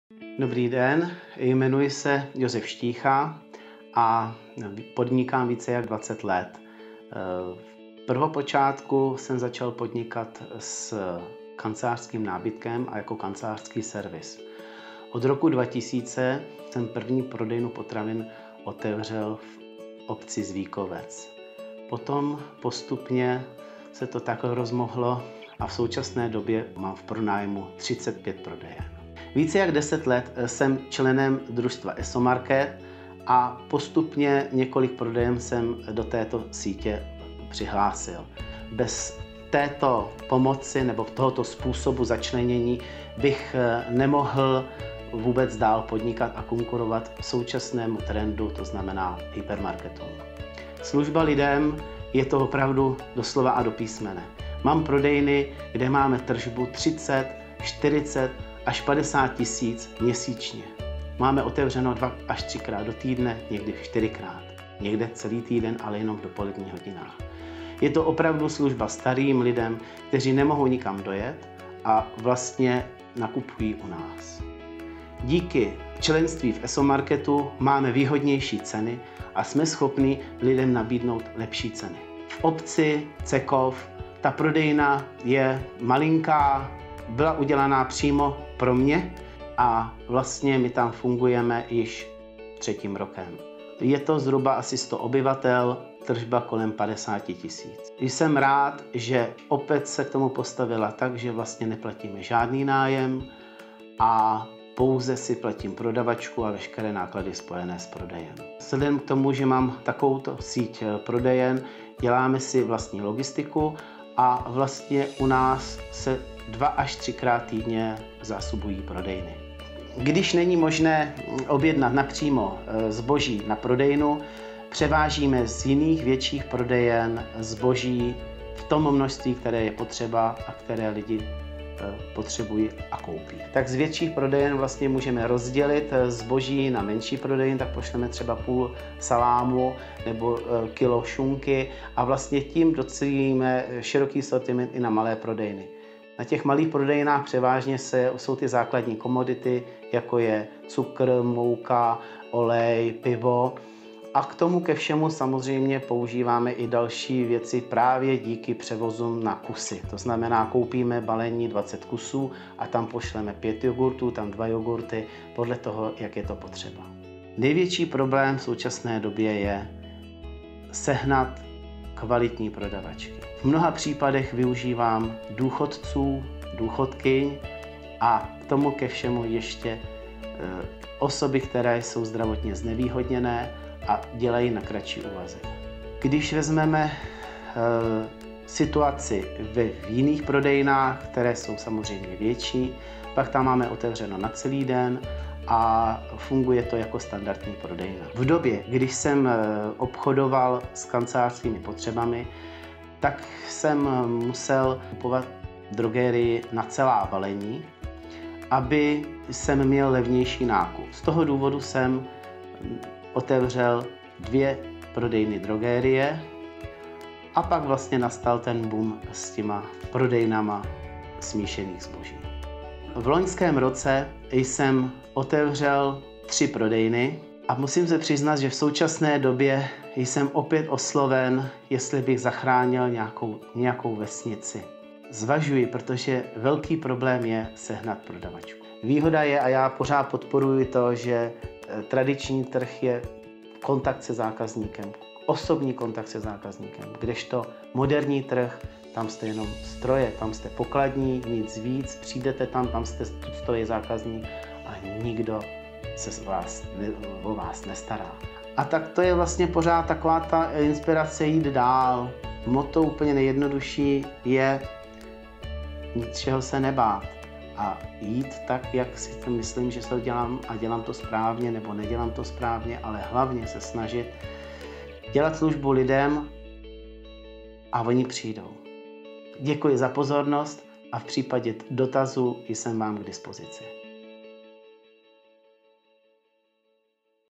Josef Štícha, majitel, ESO-CZ český obchod, Zbiroh
17. kongres Samoška, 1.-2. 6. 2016 Aquapalace Hotel Prague